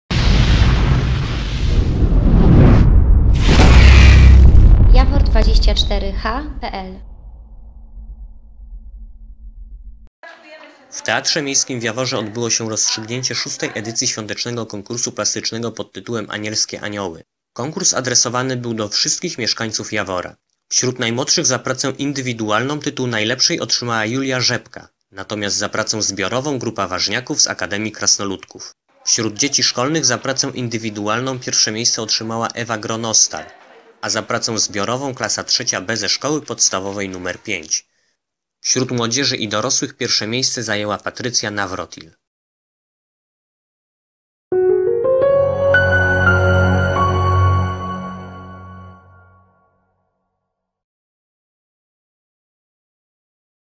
JAWOR: Rozstrzygnięcie konkursu plastycznego „Anielskie Anioły”
W Teatrze Miejskim w Jaworze odbyło się rozstrzygnięcie VI edycji świątecznego konkursu plastycznego pt. „Anielskie Anioły”.